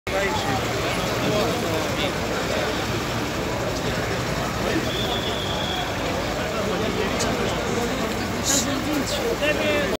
Vinçi zë poshtë punëtorin, në Korçë
Një ngjarje e rëndë është regjistruar në Korçës, pasi vinçi ka zënë poshtë një punëtor.

Ngjarja para pak minutash ne afërsi te agjensise se autobuseve.

Ende nuk dihet gjendja e punetorir te aksidentuar.